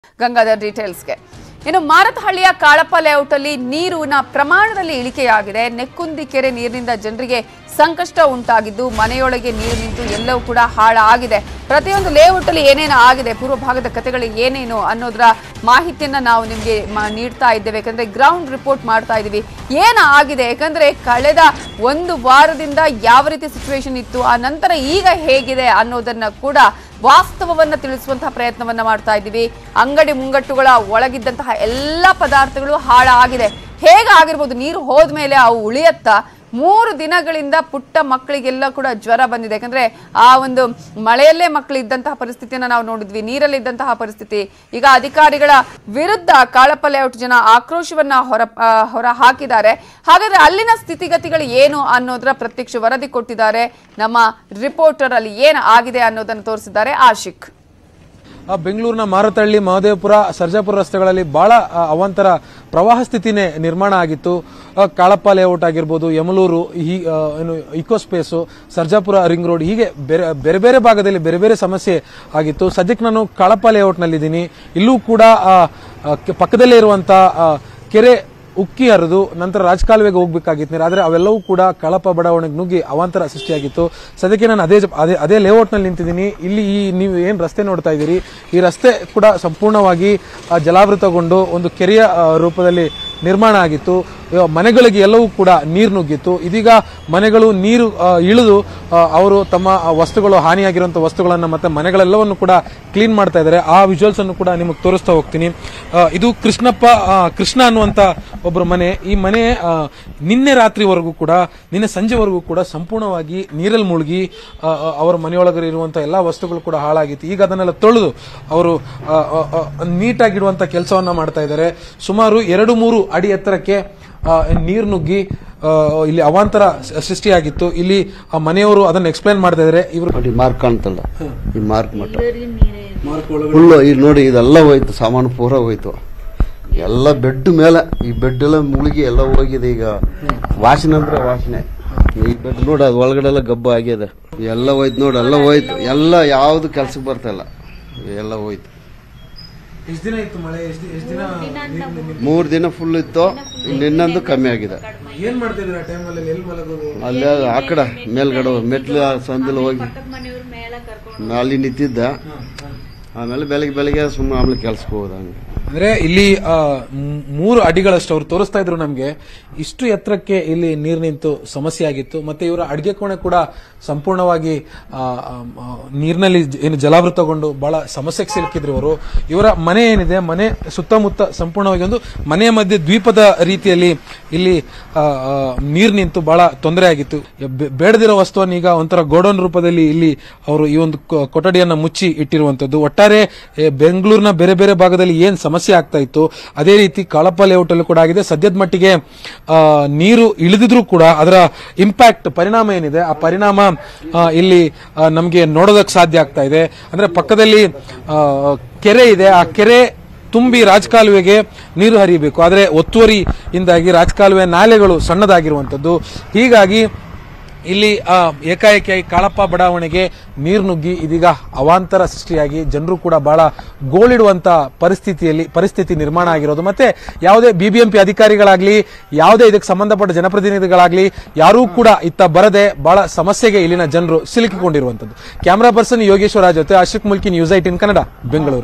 Rain In Bengaluru | Marathahalli Kalappa Layout ಜನರಿಗೆ ನೆಕ್ಕುಂದಿ ಕೆರೆ ನೀರಿನಿಂದ ಸಂಕಷ್ಟ | Kannada News
Rain Affected Areas in Bengaluru | ನೆಕ್ಕುಂದಿ ಕೆರೆ ನೀರಿನಿಂದ Marathahalli Kalappa Layout ಜನರಿಗೆ ಸಂಕಷ್ಟ | News18ರಸ್ತೆ ಕ್ಲೀನ್ ಮಾಡಲು ಅಗ್ನಿಶಾಮಕಳ ದಳ ಸಿಬಂದಿ ಹರಸಾಹಸ | Kannada News